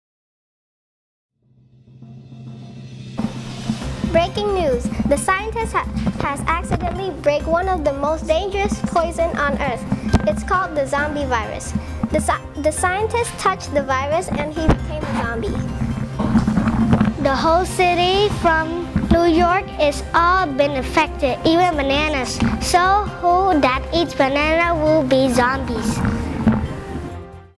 BREAKING NEWS from Junior Computer Academy STEP Cambodia!
This is a video created by students from Junior Computer Academy STEP Cambodia in the course Video Filming and Editing. They learn how to write a script, film a video and edit, even how to add special effects. And these are only 8 to 14 y.o. students.